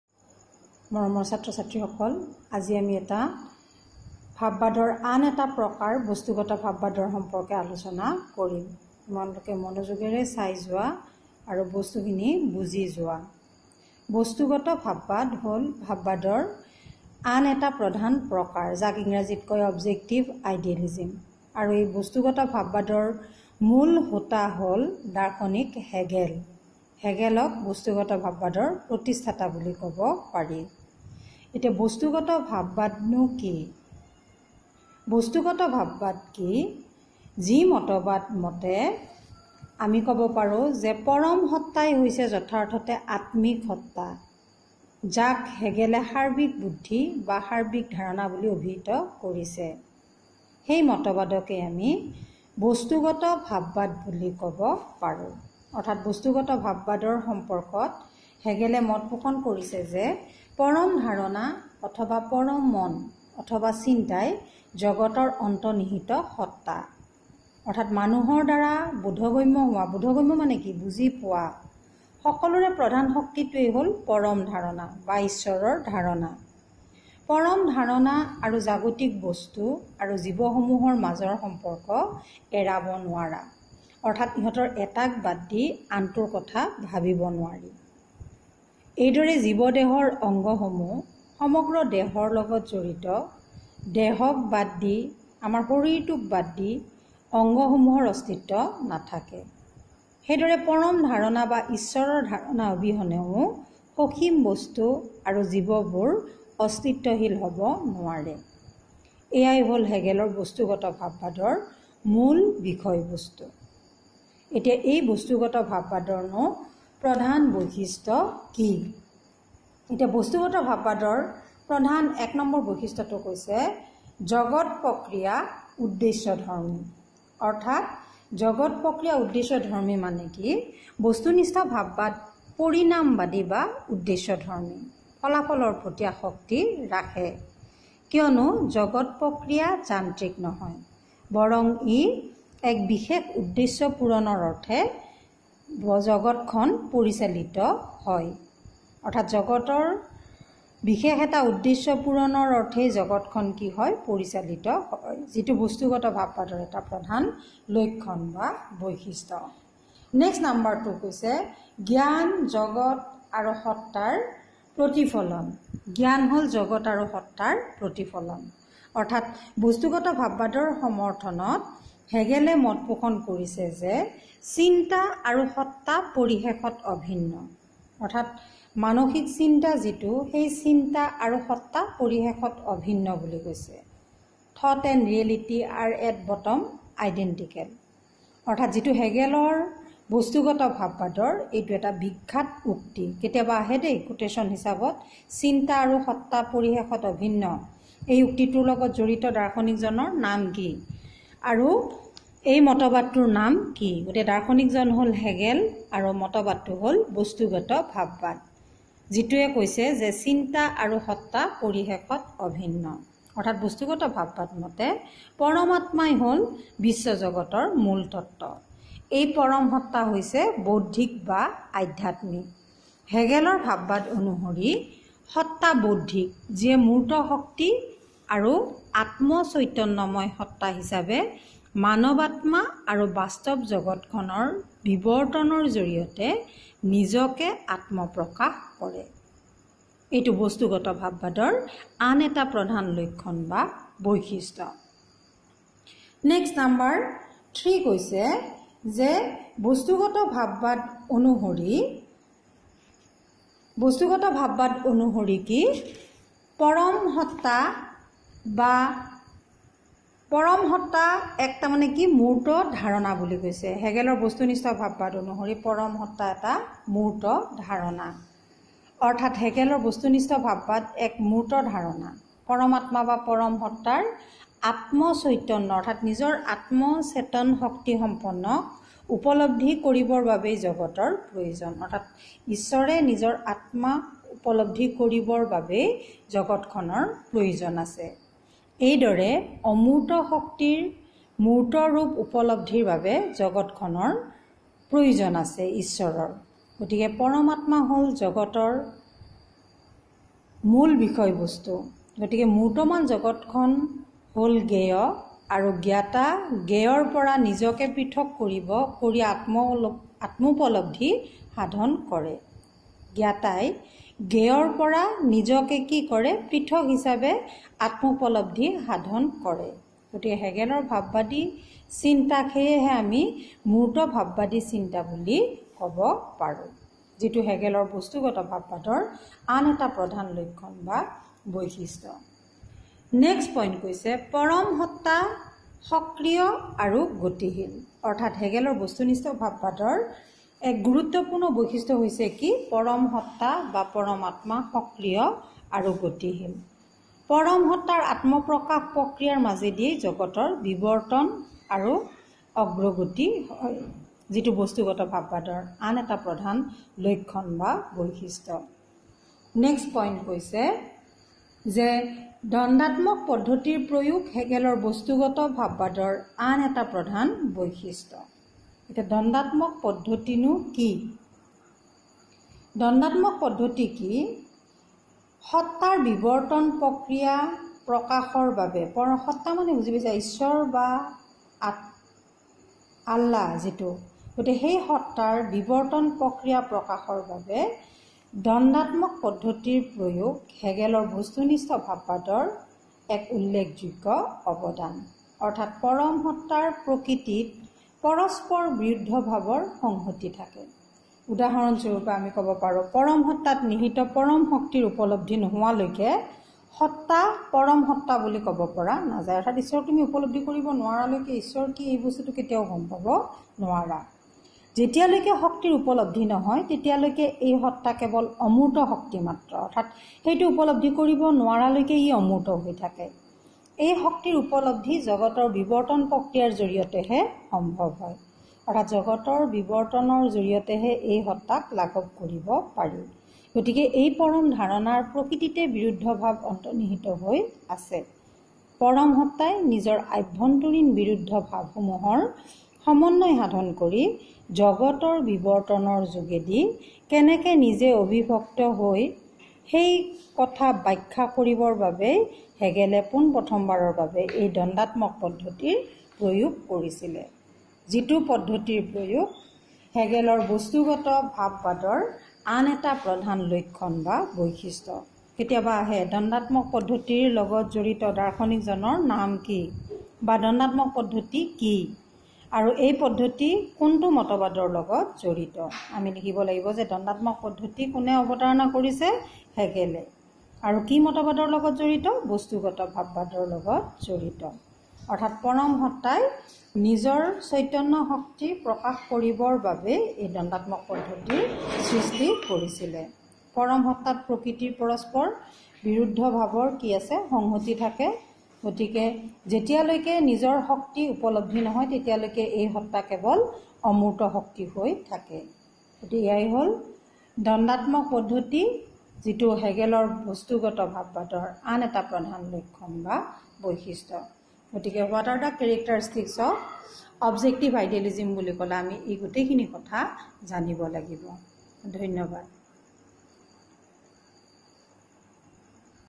Logic and Philosophy/ H.S. 2nd year/ Unit :6 Definition and Charecteristics of Objective Idealism
বস্তুগত বা বস্তুনিষ্ঠ ভাববাদৰ সংজ্ঞা আৰু বৈশিষ্ট্য